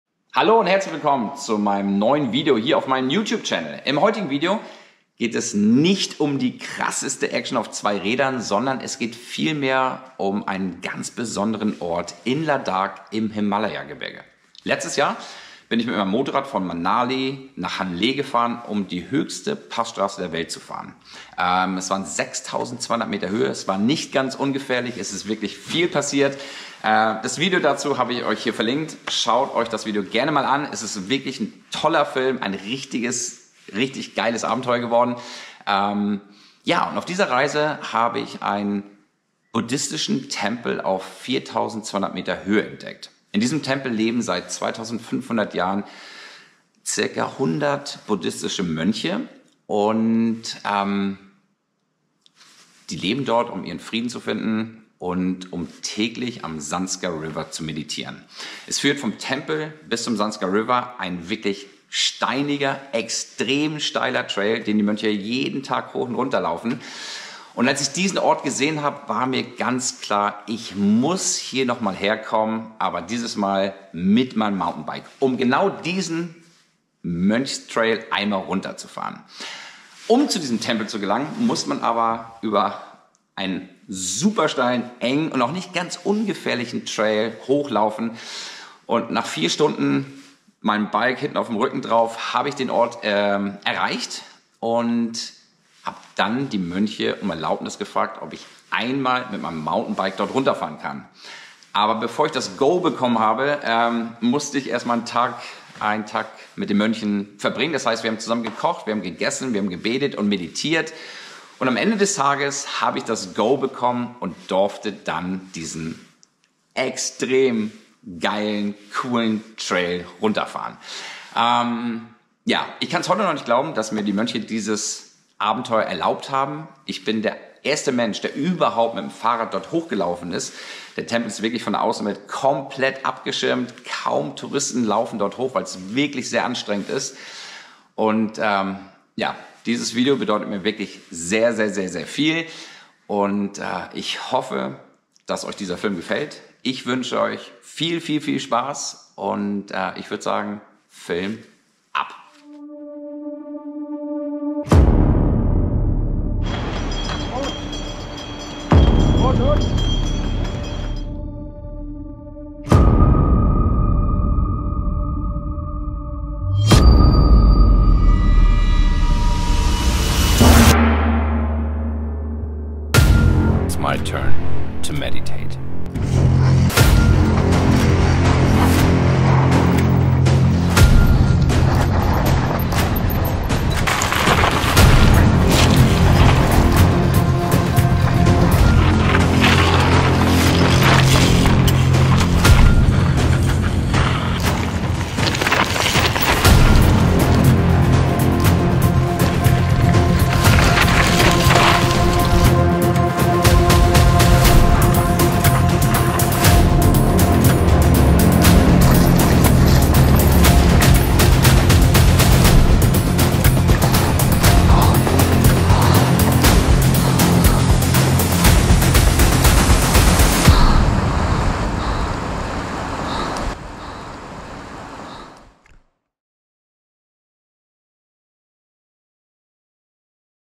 Ich bin der erste Mountainbiker der Welt der diesen Trail fahren durfte!
Ich liebe das Land Indien! Es fasziniert mich einfach, die Menschen, die Kultur, das Essen, die Religion, einfach alles ich spannend und einzigartig!

Auf meiner letzten Motorradreise durch Himalaya Gebirge habe ich wieder einen ganz besonderen Tempel auf über 4200m Höhe entdeckt. Der Tempel Phuktal Gump befindet sich in Ladahk und ist einer der ältesten Buddhistischen Tempel . Für mich war klar zu diesem magischen Ort zurück zu kehren, aber dieses mal mit meinem Mountainbike um den Trail der Mönche hinabzufahren. Es ist unglaublich, aber vor mir hat noch kein Mensch diesen steilen und steinigen Weg zum Tempel auf sich genommen um mit dem Mountainbike den Trail der Mönche zum Zanskar River hinabzufahren.

THIS IS MY WAY OF MEDITATION!